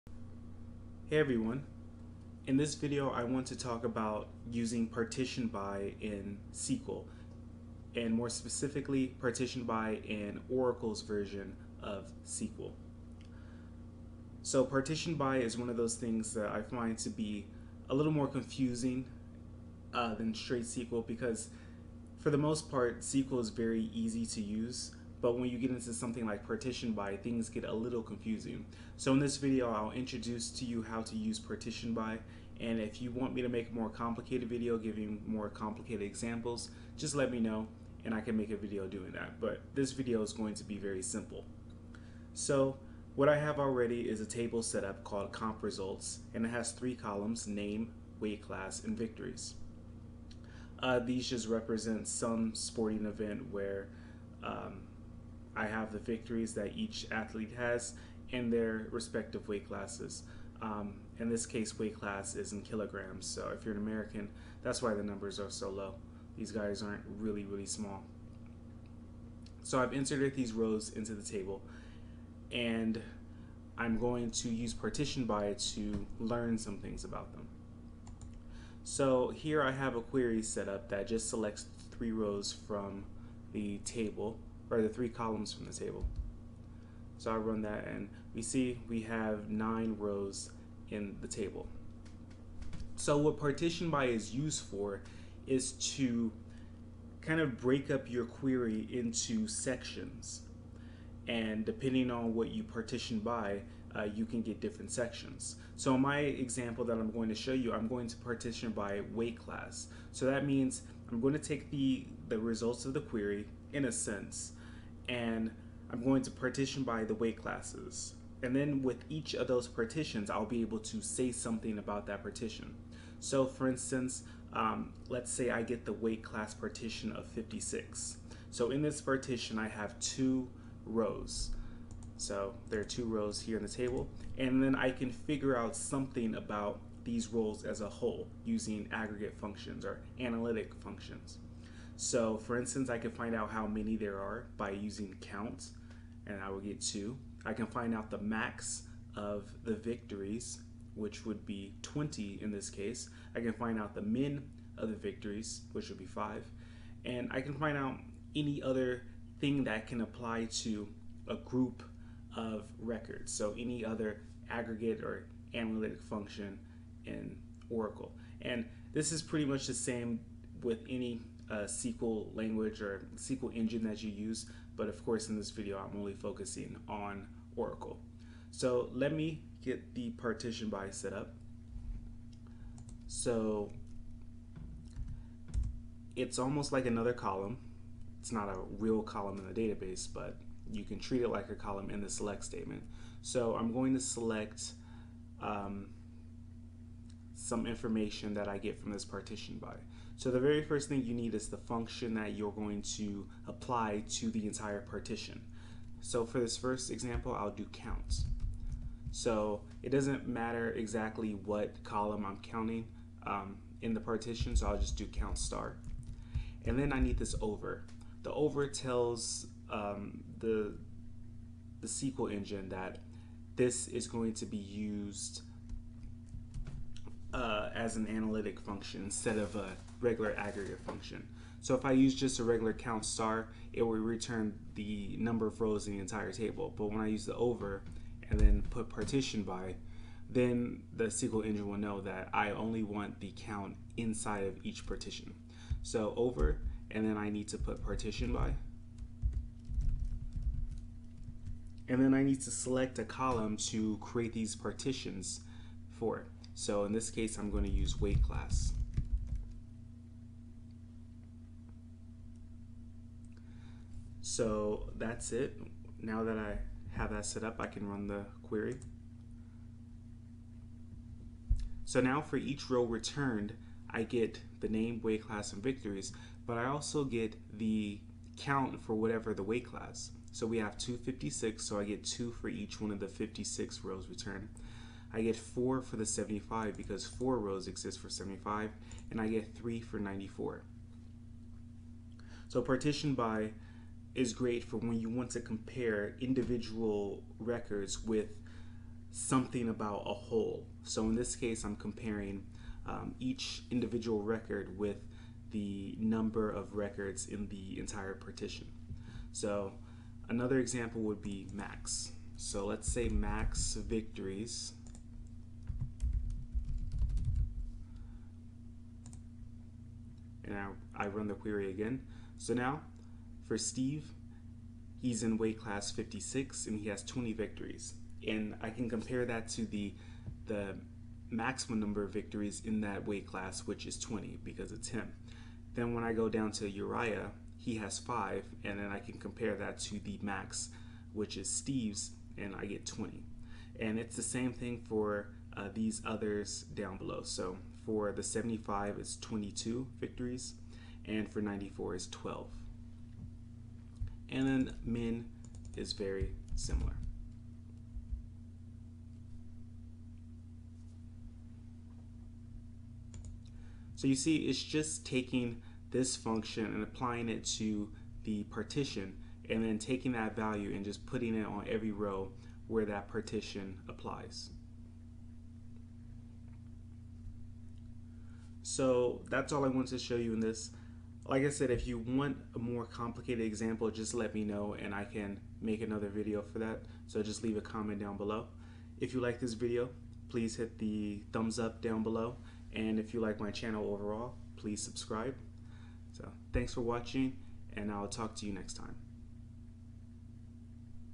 SQL Partition By Explained
This video explains how to use Partition By in Oracle's SQL, which is similar in other SQL engines with the Partition By keyword.